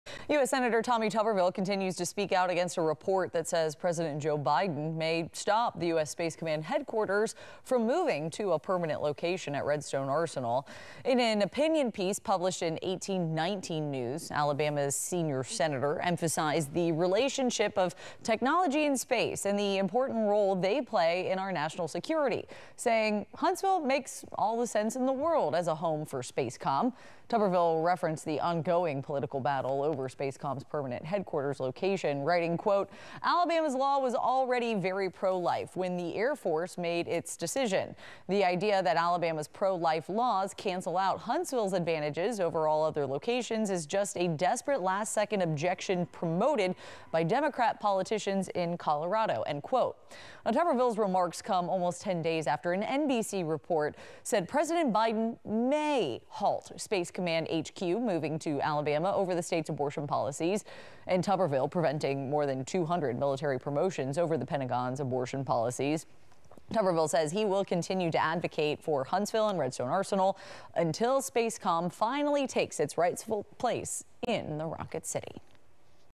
Sen. Tommy Tuberville Continues to Speak Out About Space Command in Op-Ed | May 24, 2023 | News 19 a
Senator Tommy Tuberville (R-Ala.) continues to speak out against a report that states President Joe Biden may stop Space Command Headquarters from moving to a permanent location at Redstone Arsenal.